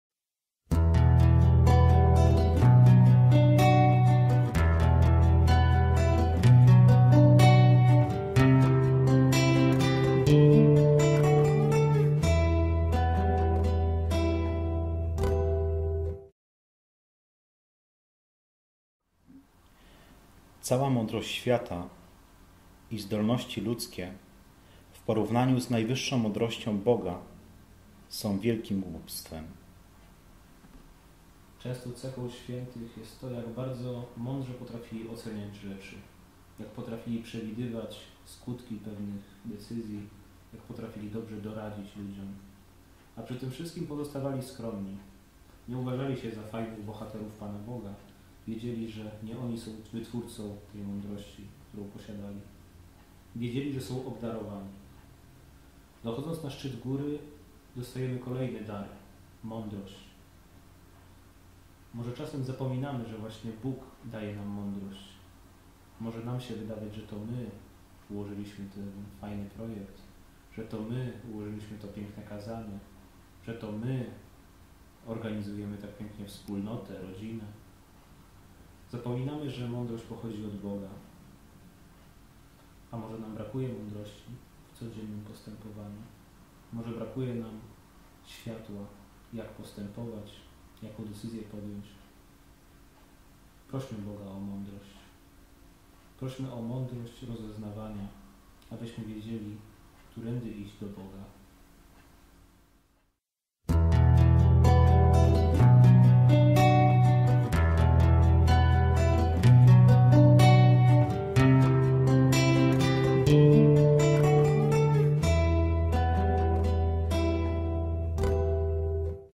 Szczyt Wszystkiego - owoce Ducha: MĄDROŚĆ (30)
Wielki Post ze św. Janem od Krzyża.
Czas poznawania siebie i życiowej wędrówki na Górę Karmel - Górę Boga - Szczyt Wszystkiego.
Prowadzą: bracia alumni Wyższego Seminarium Karmelitów Bosych w Krakowie.